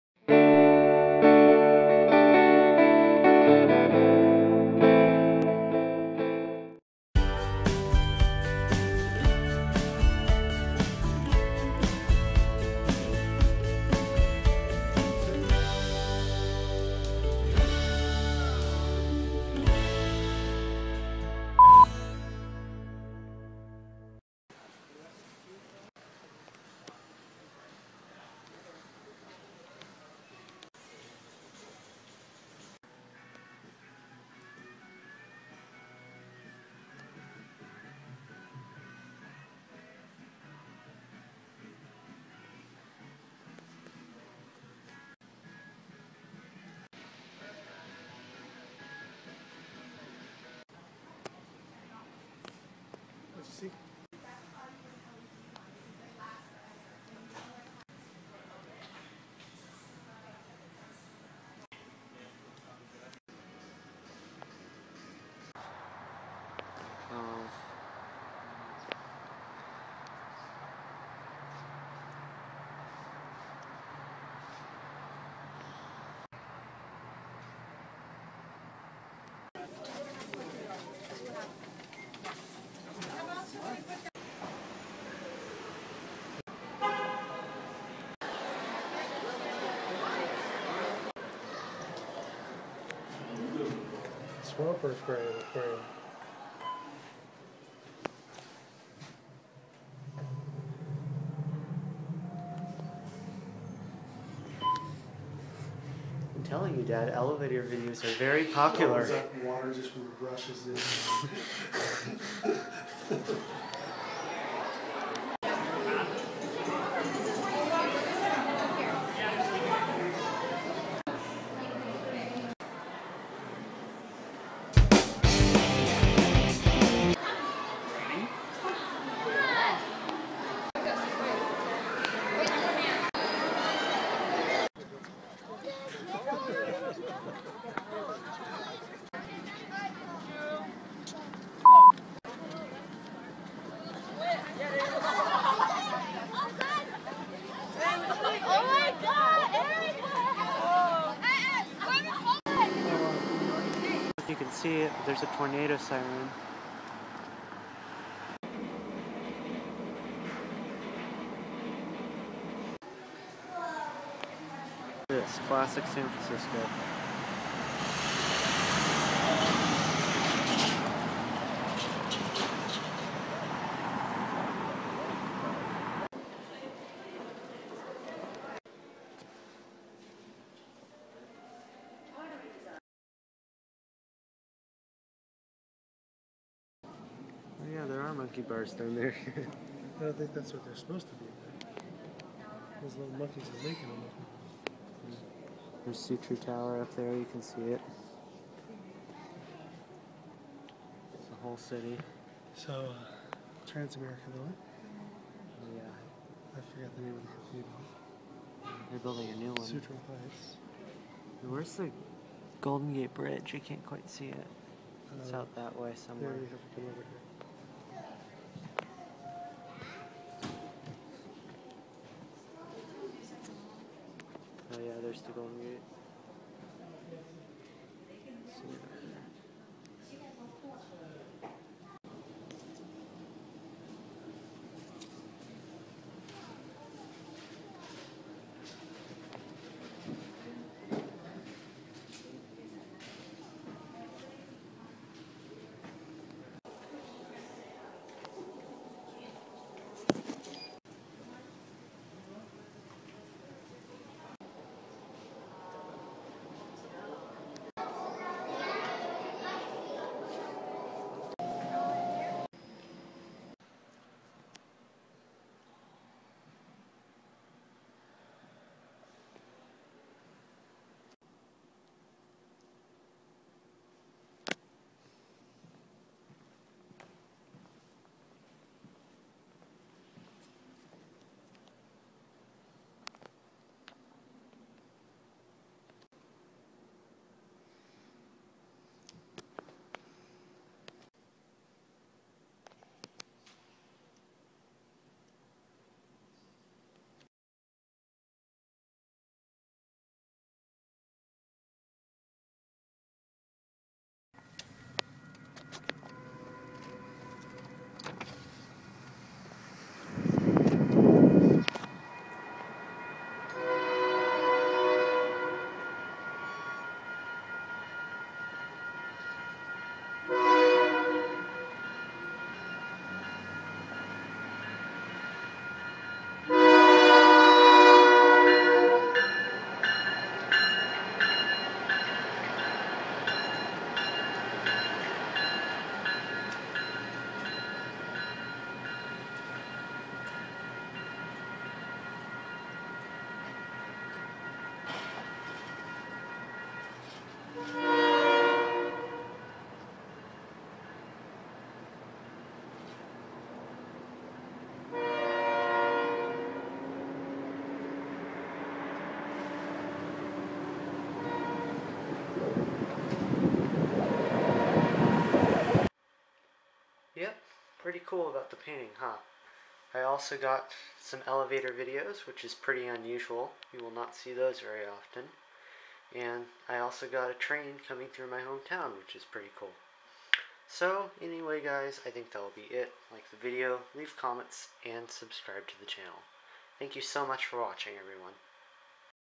Out in Wheelove Fire Alarms country! Ep. 3
Yep! This time I got a ton of footage from my trip to California! (there's some strange alarms...) I also have a picture of a very famous painting I found, an elevator video, views from the deYoung Observation tower in San Francisco and to top it off, a video of a train coming through my home town. Please enjoy! :) sorry for the beep at the start. My censoring program crashed and put one extra censor bleep in the video storyboard by mistake.